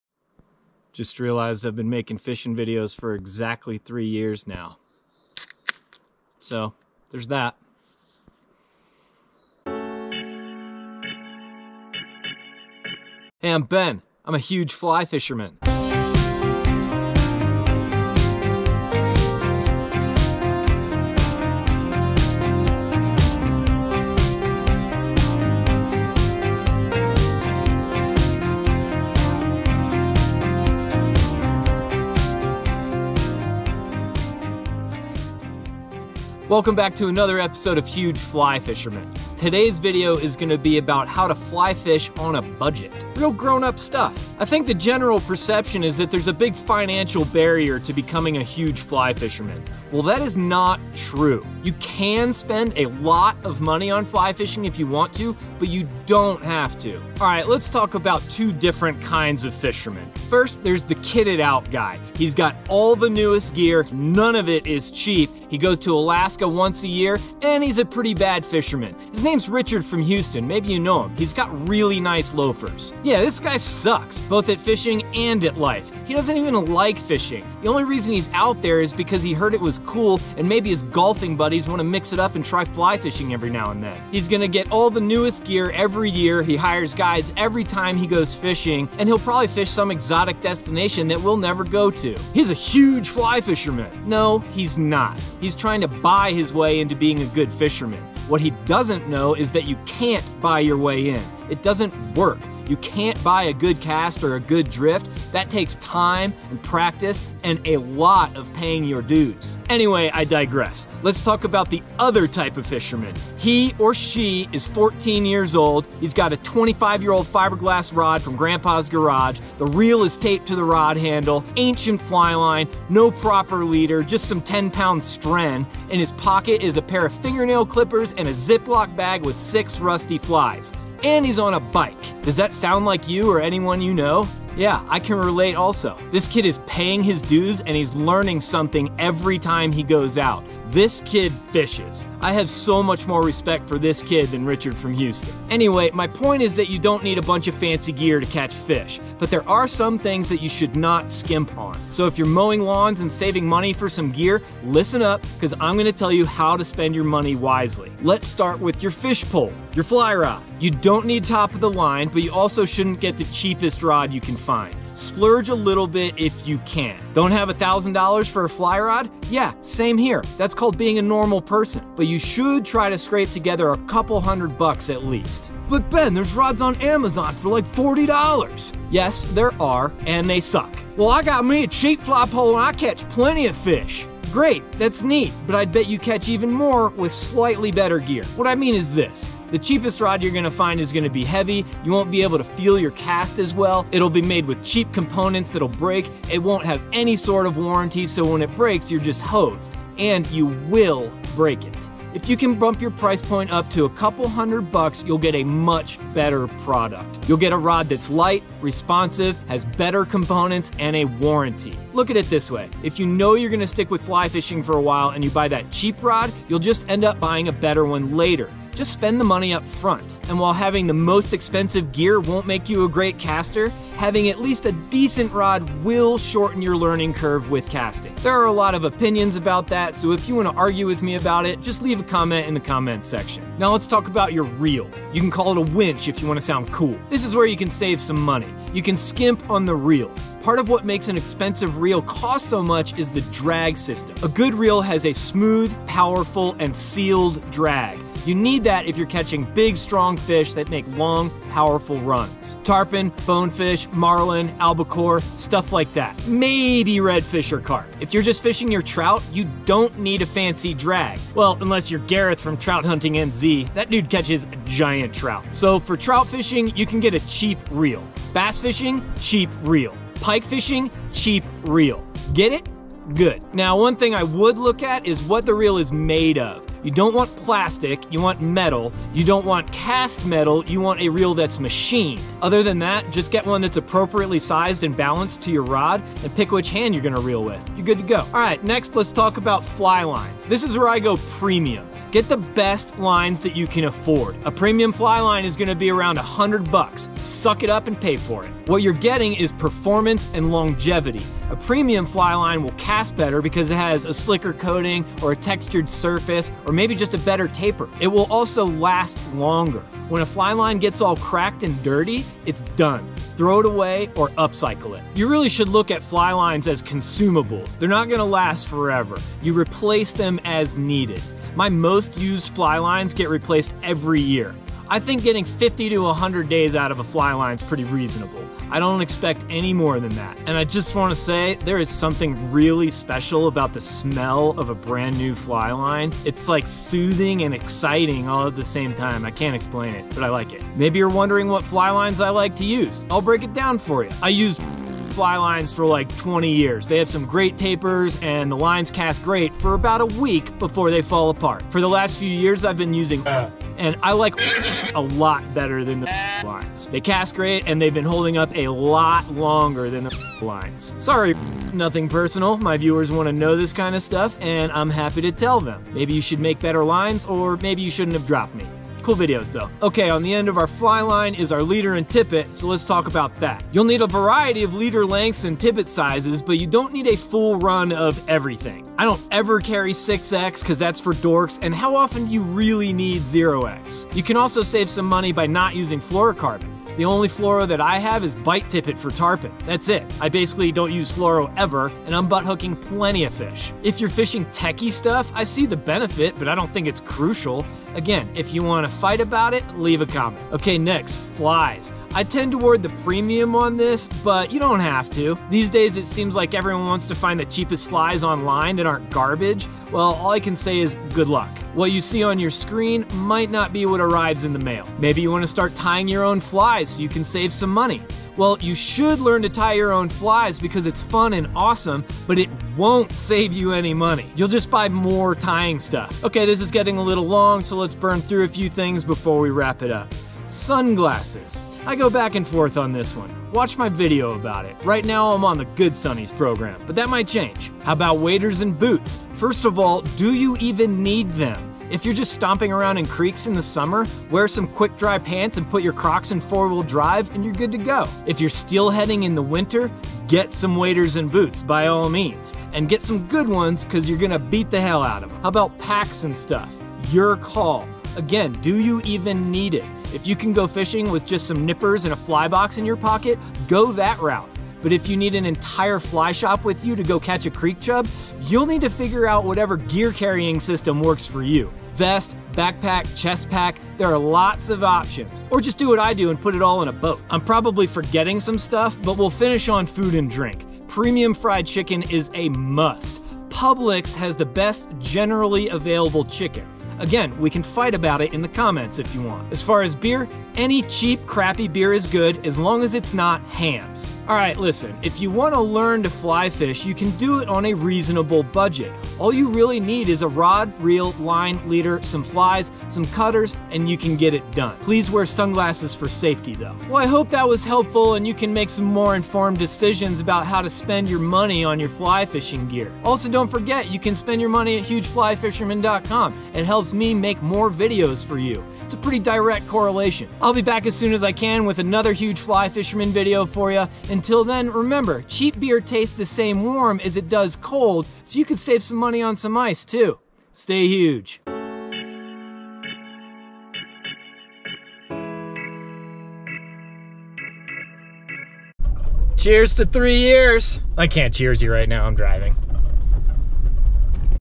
Fly Fishing on a BUDGET
If you're not sure where to put your money, I'll break it down for you.

friends

bimini for raft
tenkara pole
Smith sunglasses
Costa sunglasses
water cannon
streamer line
trout book
kids fly rod
fly tying tool kit